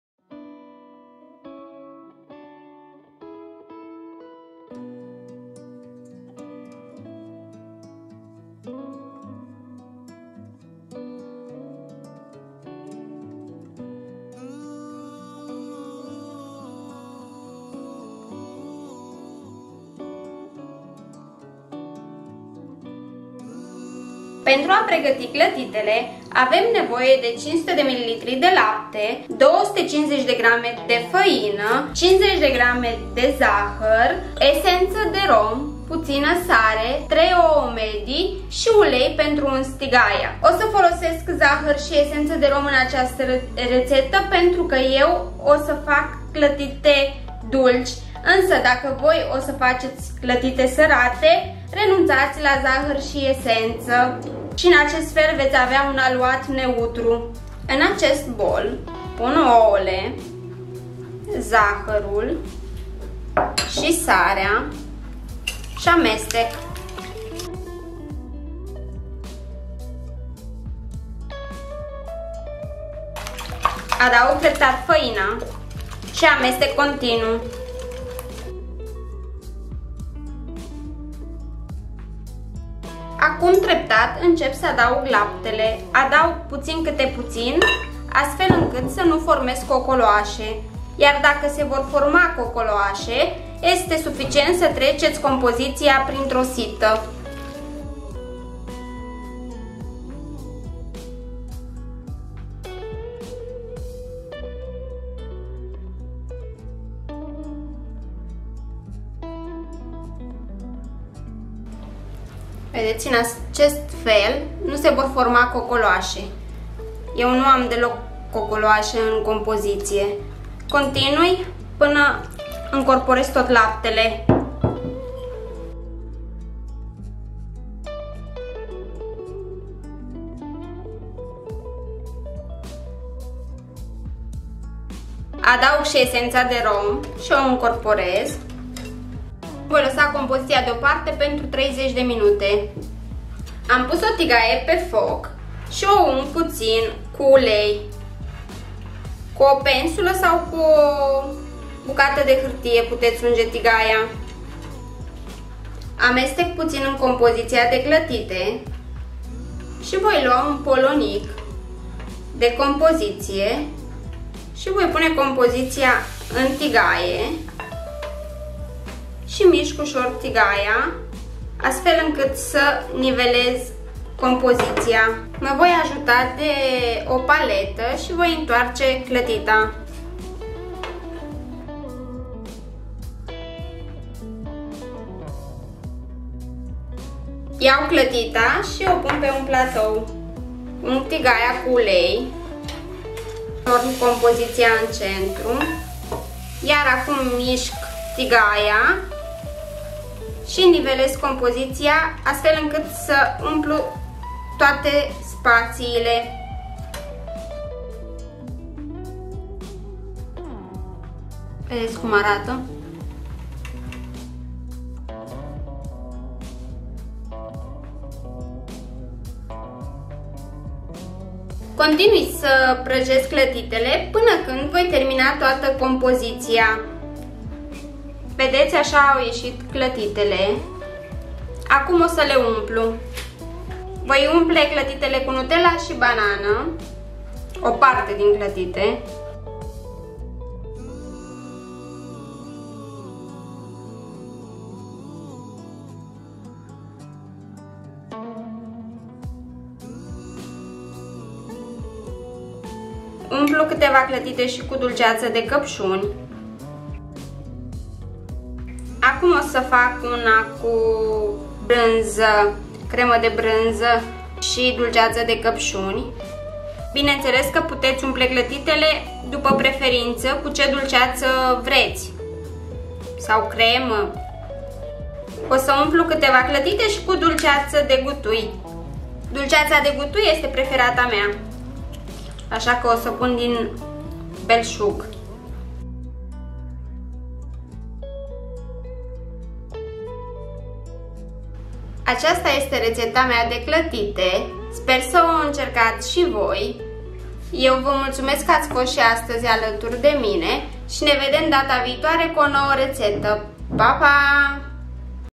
Clătite rețeta simplă pas cu pas | Maria Popescu
Reteta de astazi este despre si cu clatite, mie clatitele imi aduc aminte de copilarie, cand am invatat sa fac si prima oara clatite. nu erau ele perfecte dar erau cele mai bune , imi amintesc si de adolescenta cand faceam asa multe clatite care dispareau asa repede de pe platou incat nu se adunau niciodata mai mult de 3,tot in adolescenta am facut pentru prima data clatite iubitului meu, ce de amintiri despre si cu clatite.
Ingrediente:500ml lapte,250g faina,50g zahar,esenta rom,putina sare,3 oua,ulei pentru uns tigaia.